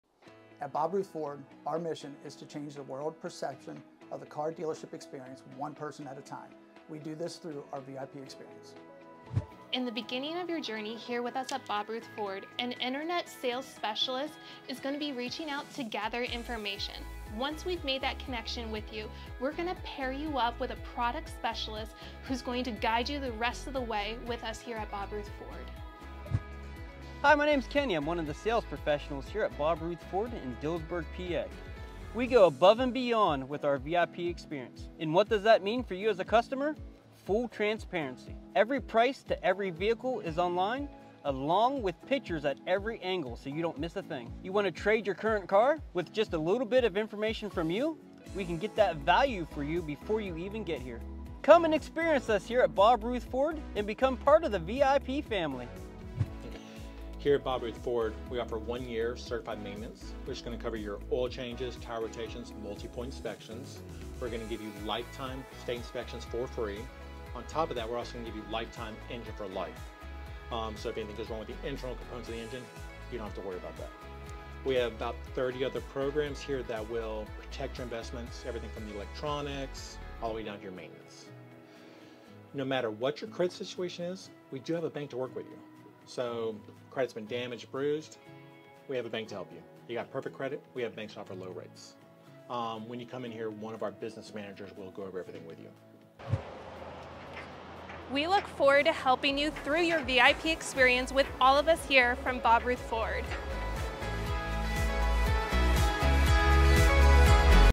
The Bob Ruth Ford VIP Experience - what is it?
Take a virtual walk through the Bob Ruth Ford VIP Experience... follow us this way!